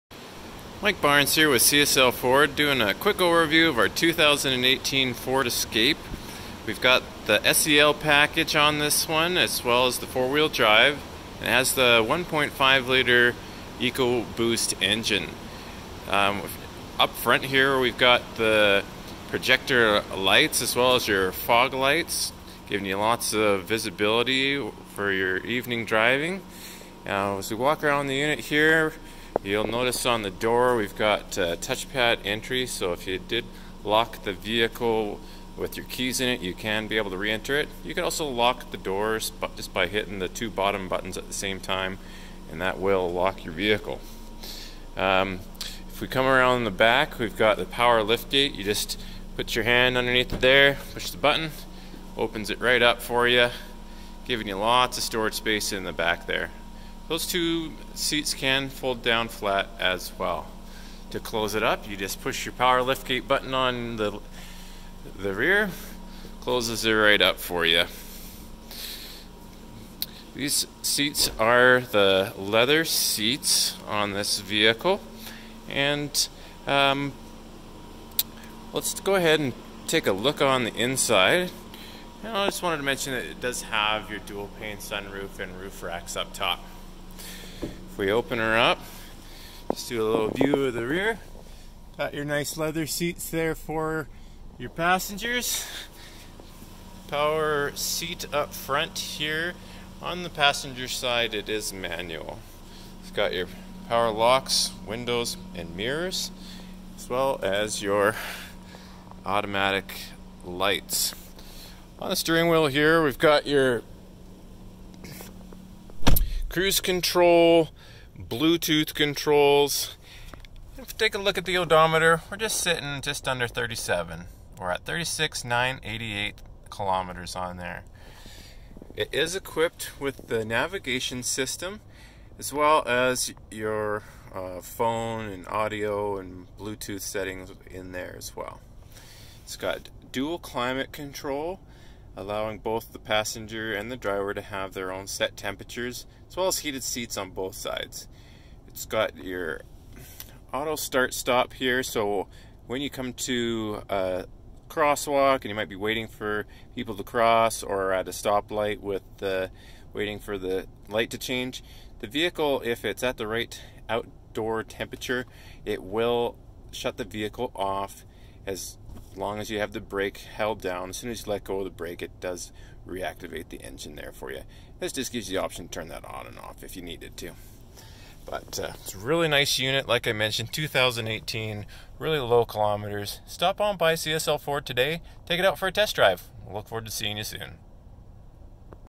2018 Ford Escape SEL, 1.5L EcoBoost, 4x4, SunRoof, Nav
This is a overview video of the 2018 Ford Escape SEL that we have at the store.
Very lightly used. Has 4x4, SunRoof and navigation system.
Stop by CSL Ford and take it for a test drive.